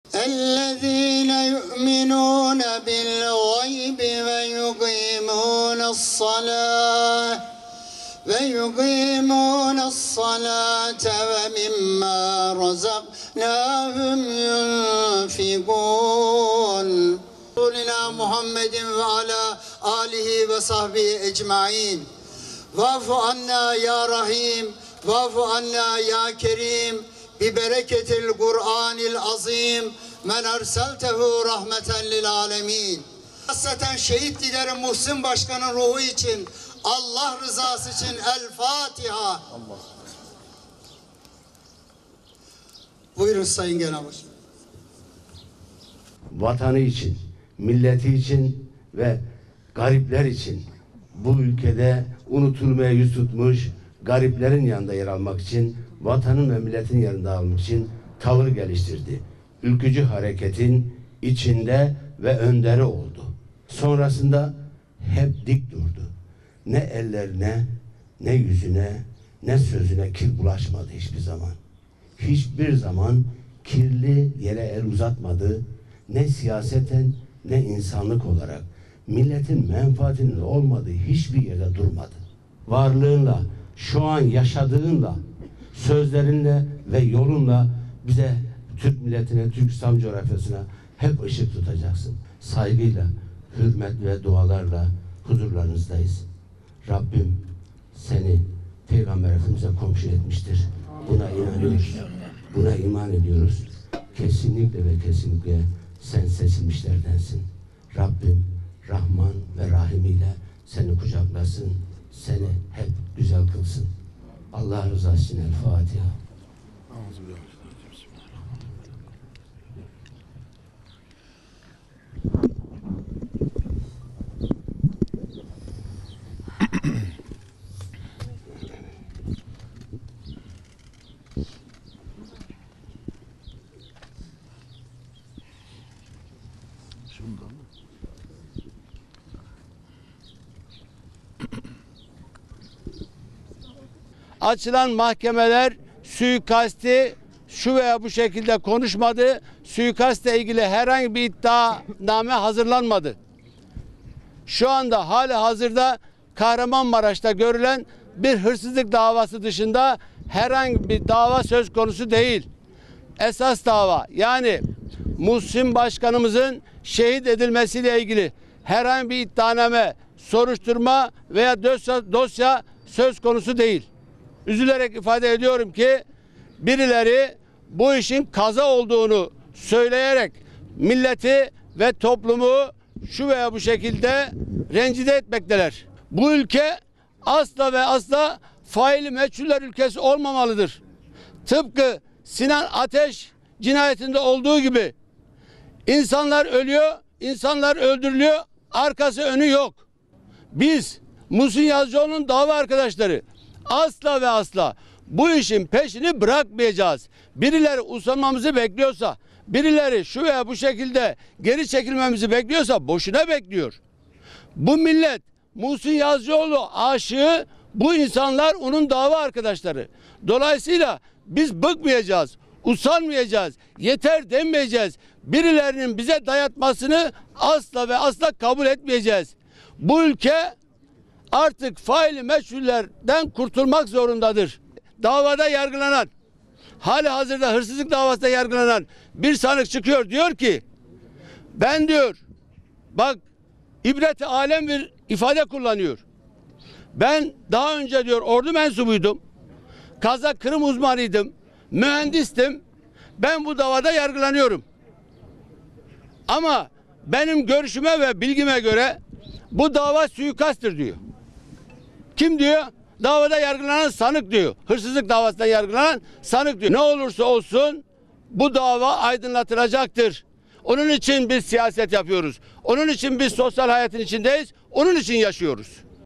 Milli Yol Partisi, Muhsin Yazıcıoğlu'nu Kabri Başında Andı
MİLLİ Yol Partisi, Büyük Birlik Partisi (BBP) Kurucu Genel Başkanı Muhsin Yazıcıoğlu'nu, ölümünün 14'üncü yıl dönümünde kabri başında andı.

Kahramanmaraş’ın Göksun ilçesinde, 25 Mart 2009'da helikopter kazası sonucu beraberinde 5 kişi ile hayatını kaybeden BBP Kurucu Genel Başkanı Muhsin Yazıcıoğlu için Taceddin Dergahı'ndaki kabri başında anma töreni düzenlendi. Törene Milli Yol Partisi Genel Başkanı Remzi Çayır ve partililer ile kardeşi Yusuf Yazıcıoğlu da katıldı. Anma töreninde yapılan Kur'an-ı Kerim tilavetinin ardından Çayır ve partililer Yazıcıoğlu'nun kabrine karanfil bırakarak, dua etti.